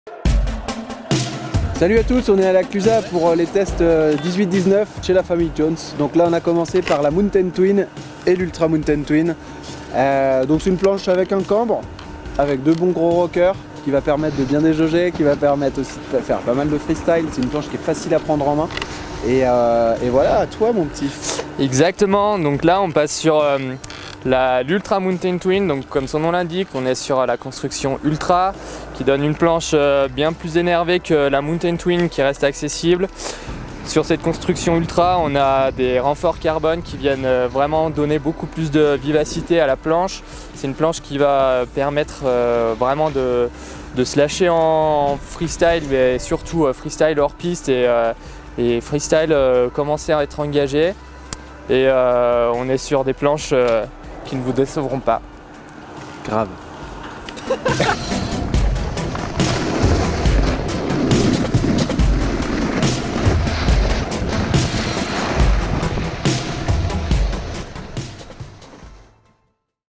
Jones : Mountain Twin & Ultra Mountain Twin 2019
La Mountain Twin est une planche à destination des riders de niveau intermédiaire à confirmé qui veulent une planche polyvalente avec un caractère très joueur pour pouvoir lancer quelques tricks autant sur les kickers du park que sur les pillows gavés de neige!

La Mountain Twin ressemble dans ses capacités à l'Ultra Mountain Twin mais légèrement moins incisive que sa grande soeur. La Mountain Twin est plus souple en torsion que la version Ultra ce qui la rend plus agréable sur les réceptions parfois un peu trop appuyées.